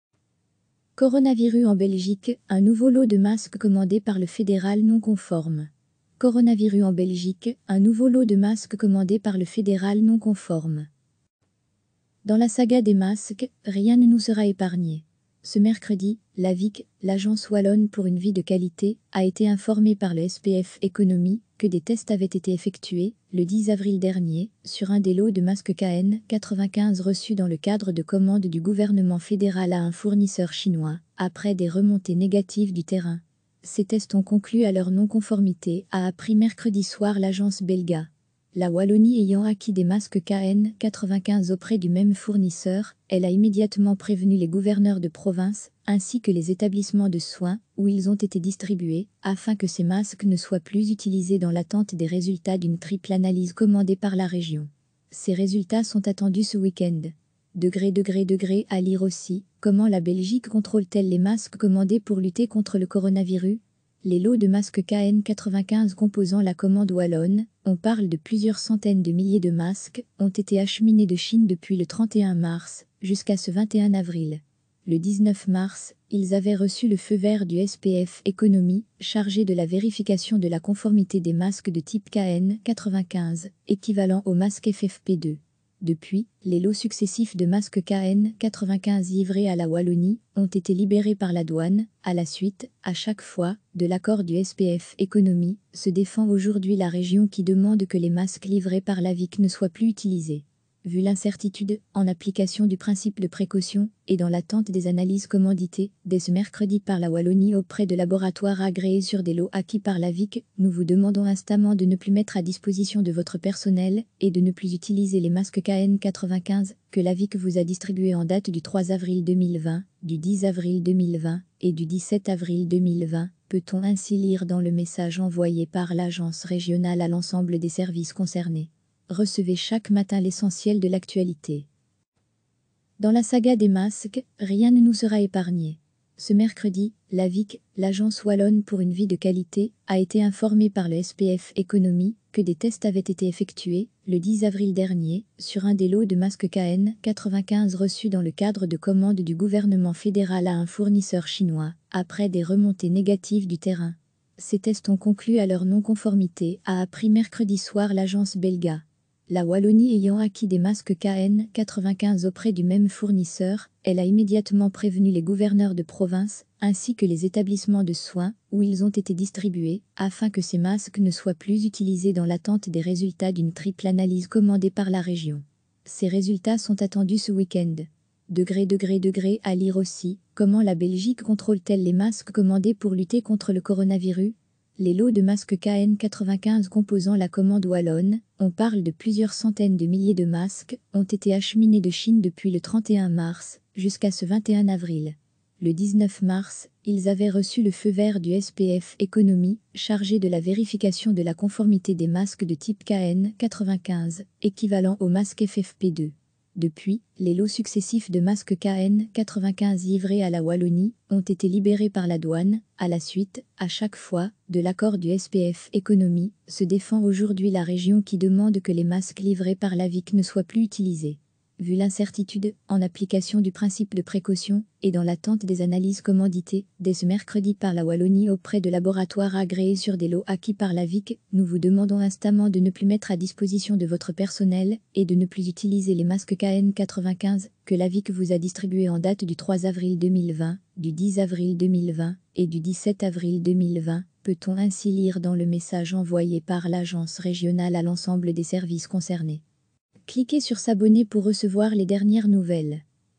Coronavirus en Belgique : un nouveau lot de masques commandés par le Fédéral non-conformes
Dans la saga des masques, rien ne nous sera épargné. Ce mercredi, l’Aviq, l’agence wallonne pour une vie de qualité, a été informée par le SPF Economie que des tests avaient été effectués, le 10 avril dernier, sur un des lots de masques KN 95 reçus dans le cadre de commandes du gouvernement fédéral à un fournisseur chinois, après des remontées négatives du terrain. Ces tests ont conclu à leur non-conformité, a appris mercredi soir l’agence Belga.

La Wallonie ayant acquis des masques KN 95 auprès du même fournisseur, elle a immédiatement prévenu les gouverneurs de province ainsi que les établissements de soins où ils ont été distribués afin que ces masques ne soient plus utilisés dans l’attente des résultats d’une triple analyse commandée par la Région. Ses résultats sont attendus ce week-end.

►►► À lire aussi : Comment la Belgique contrôle-t-elle les masques commandés pour lutter contre le coronavirus ?

Les lots de masques KN95 composant la commande wallonne – on parle de plusieurs centaines de milliers de masques – ont été acheminés de Chine depuis le 31 mars jusqu’à ce 21 avril.

Le 19 mars, ils avaient reçu le feu vert du SPF Economie, chargé de la vérification de la conformité des masques de type KN 95, équivalents aux masques FFP2. Depuis, les lots successifs de masques KN 95 livrés à la Wallonie ont été libérés par la Douane, à la suite, à chaque fois, de l’accord du SPF Economie, se défend aujourd’hui la Région qui demande que les masques livrés par l’AViQ ne soient plus utilisés.

"Vu l’incertitude, en application du principe de précaution et dans l’attente des analyses commanditées dès ce mercredi par la Wallonie auprès de laboratoires agréés sur des lots acquis par l’AViQ, nous vous demandons instamment de ne plus mettre à disposition de votre personnel et de ne plus utiliser les masques KN95 que l’AViQ vous a distribués en date du 3 avril 2020, du 10 avril 2020 et du 17 avril 2020", peut-on ainsi lire dans le message envoyé par l’agence régionale à l’ensemble des services concernés.